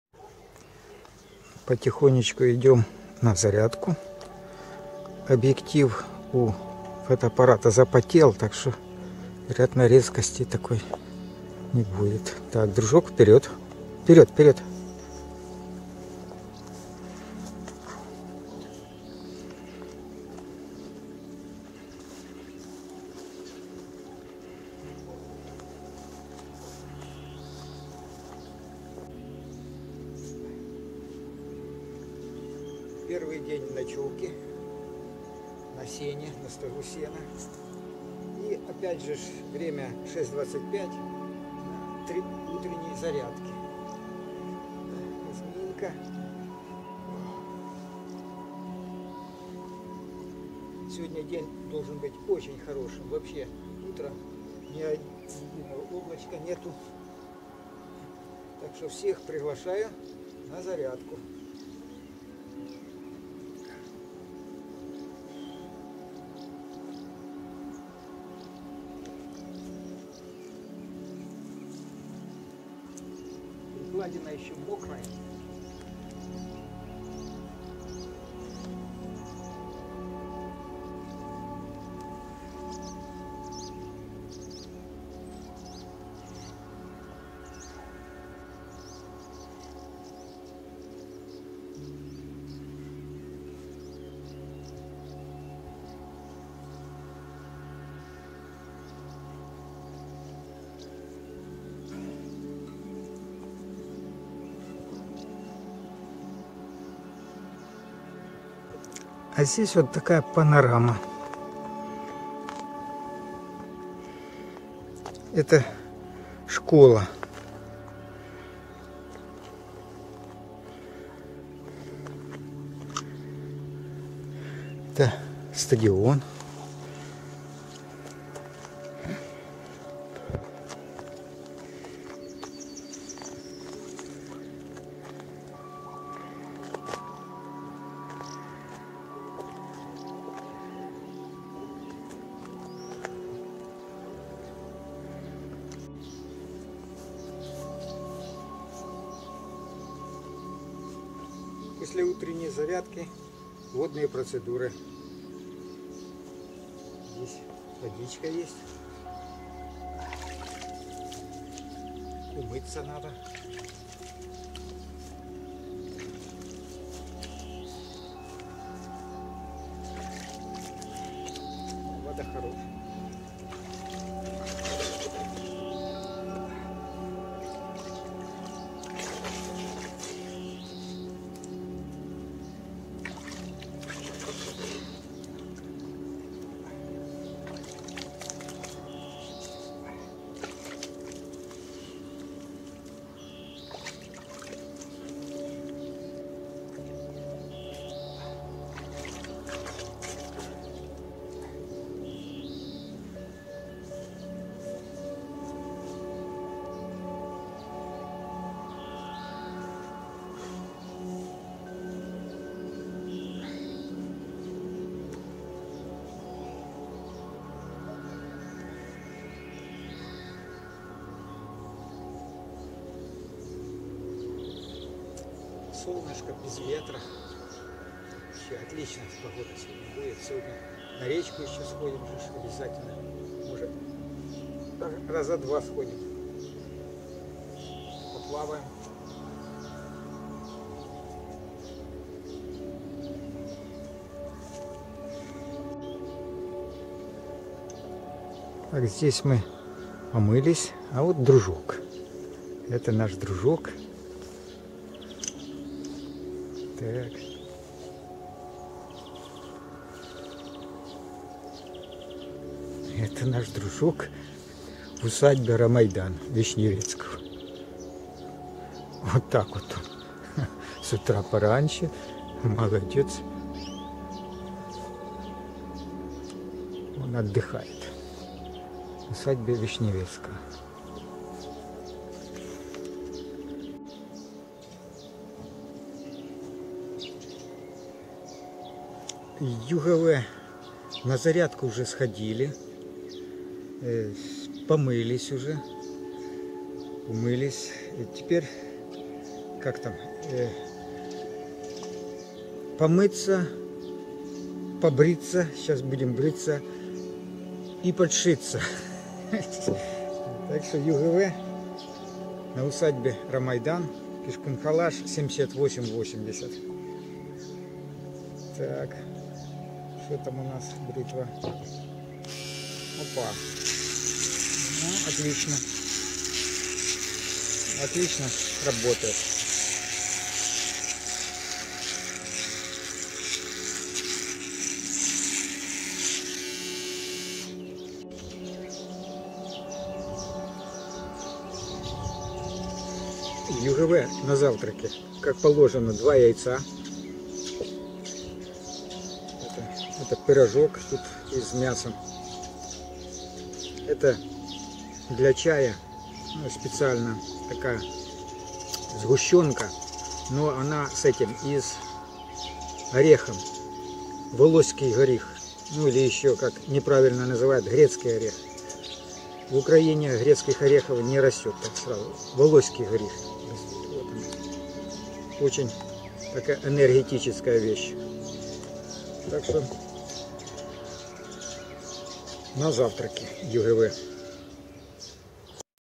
ЮГВ зарядка, умывание, завтрак - на полевом выходе Кишкунхалаш 78-80г.
Утром велосипедом прибыл в село Хорошки, Лубенский район, усадьба Вишневецкого Рамайдан ... всё начинается с зарядки и дальше по плану ...